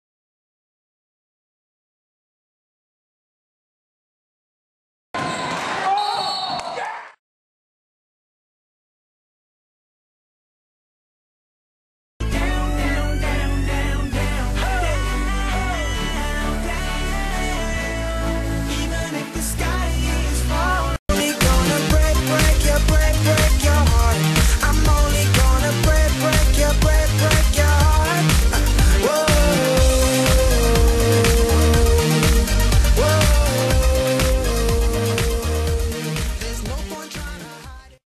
The funniest flop Zac has ever done!
The WVLHS senior takes an epic charge against his buddy! it was the funniest charge he's ever taken and it was a total flop. A hint to anyone who wants to take a charge, just grunt and jump backwards and you might get the call like he did. It just might work. :)